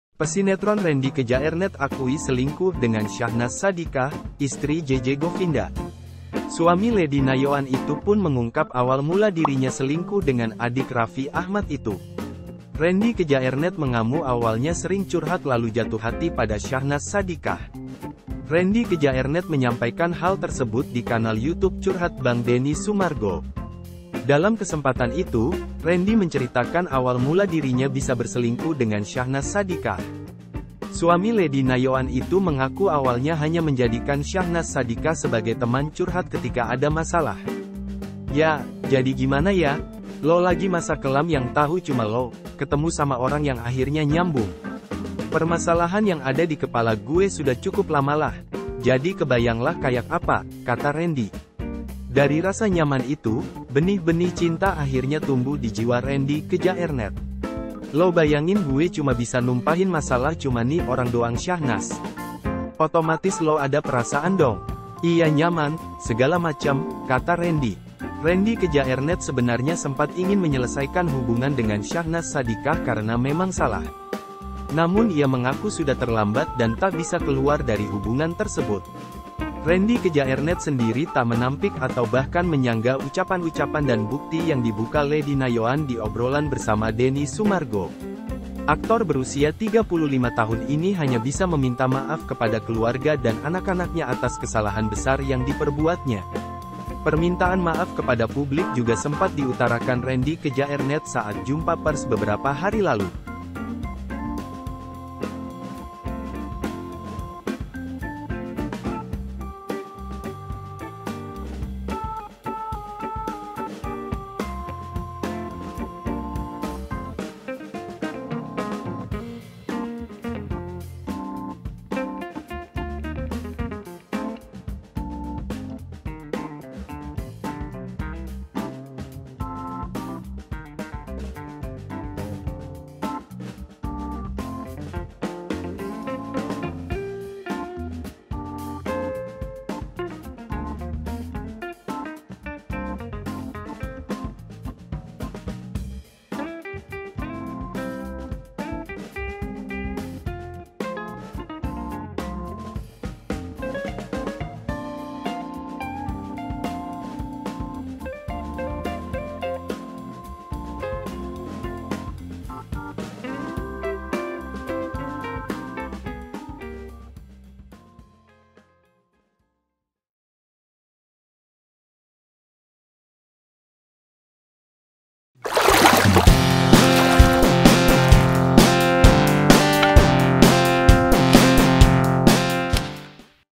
Rendy Kjaernett Ungkap Awal Mula Selingkuh dengan Syahnaz Sadiqah
Pesinetron Rendy Kjaernett akui selingkuh dengan Syahnaz Sadiqah, istri Jeje Govinda.

Suami Lady Nayoan itu pun mengungkap awal mula dirinya selingkuh dengan adik Raffi Ahmad itu.

Rendy Kjaernett mengamu awalnya sering curhat  lalu jatuh hati pada Syahnaz Sadiqah.

Rendy Kjaernett menyampaikan hal tersebut di kanal YouTube Curhat Bang Denny Sumargo.

Dalam kesempatan itu, Rendy menceritakan awal mula dirinya bisa berselingkuh dengan Syahnaz Sadiqah.

Suami Lady Nayoan itu mengaku awalnya hanya menjadikan Syahnaz Sadiqah sebagai teman curhat ketika ada masalah.

"Ya, jadi gimana ya. Lo lagi masa kelam yang tahu cuma lo, ketemu sama orang yang akhirnya nyambung. Permasalahan yang ada di kepala gue sudah cukup lamalah, jadi kebayanglah kayak apa," kata Rendy.

Dari rasa nyaman itu, benih-benih cinta akhirnya tumbuh di jiwa Rendy Kjaernett.

"Lo bayangin gue cuma bisa numpahin masalah cuma nih orang doang Syahnaz. Otomatis lo ada perasaan dong. Iya nyaman, segala macem," kata Rendy.

Rendy Kjaernett sebenarnya sempat ingin menyelesaikan hubungan dengan Syahnaz Sadiqah karena memang salah.

Namun ia mengaku sudah terlambat dan tak bisa keluar dari hubungan tersebut.

Rendy Kjaernett sendiri tak menampik atau bahkan menyanggah ucapan-ucapan dan bukti yang dibuka Lady Nayoan di obrolan bersama Denny Sumargo.

Aktor berusia 35 tahun ini hanya bisa meminta maaf kepada keluarga dan anak-anaknya atas kesalahan besar yang diperbuatnya.

Permintaan maaf kepada publik juga sempat diutarakan Rendy Kjaernett saat jumpa pers beberapa hari lalu.

HAI PARAPENDENGAR!